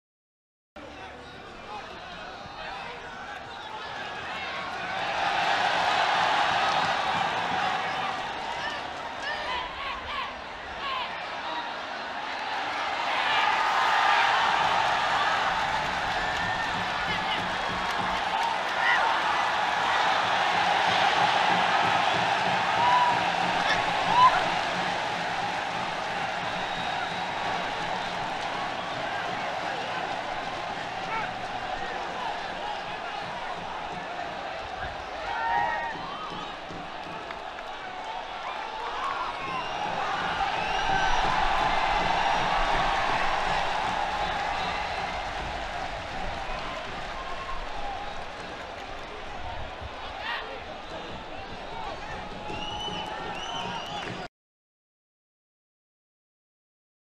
Ref overrules assistant to allow Suzuki Cup goal - Sportdec
There was a moment of controversy in Malaysia's Suzuki Cup clash with Cambodia on Sunday. Cambodia's opening goal was flagged offside by the assistant referee but overruled by the referee who allowed it to stand after some discussion.